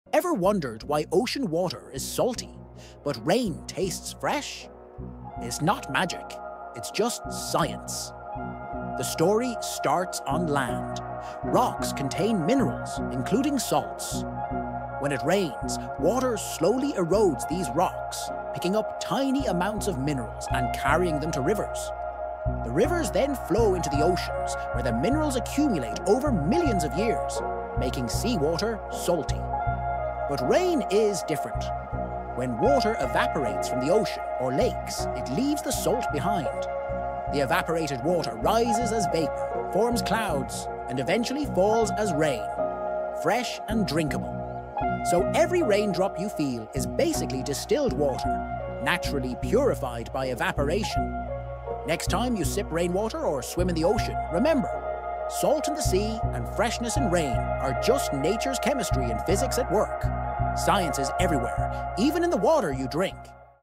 Why is sea water salty but rain is not?
Ever wondered why ocean water is salty, but rain tastes fresh? It’s not magic — it’s just science.